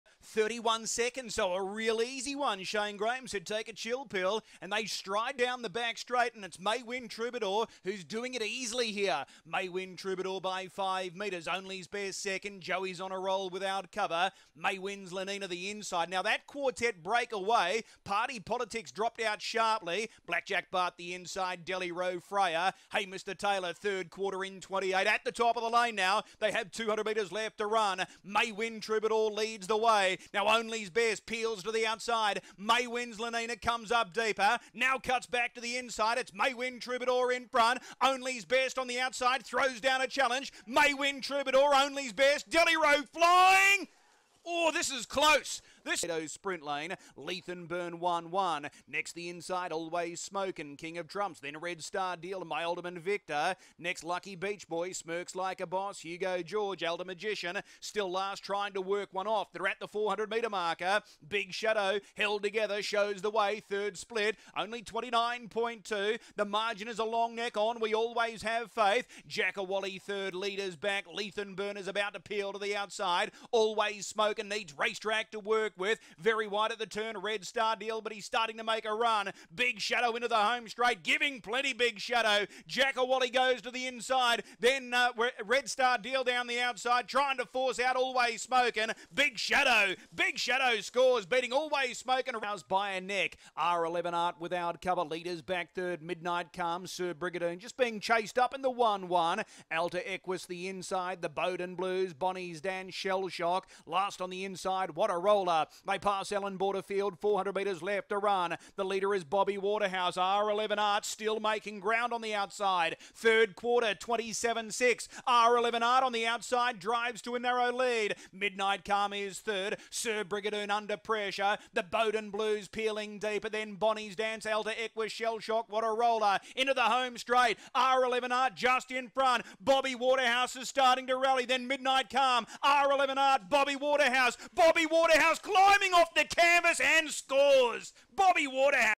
Sectional Stars & Blackbookers - March 8
Check out Darren Clayton's sectional stars and black bookers ahead of this weekend's harness racing action.